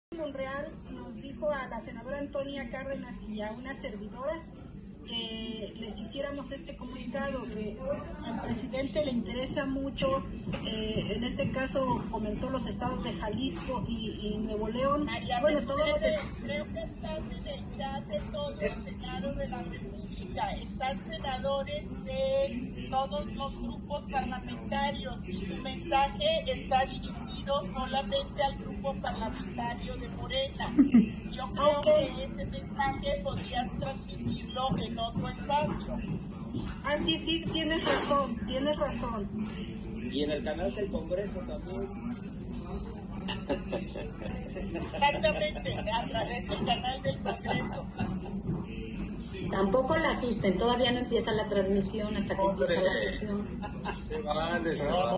Senadora de Morena se balconea
María Merced González González, senadora por Morena, no cerró su micrófono durante una reunión virtual de comisiones y se escuchó cuando dijo que el presidente Andrés Manuel López Obrador le pidió a Ricardo Monreal, presidente de la Junta de Coordinación Política, acudir a hacer campaña a Jalisco y a Nuevo León, “porque son dos estados que le interesan mucho electoralmente”.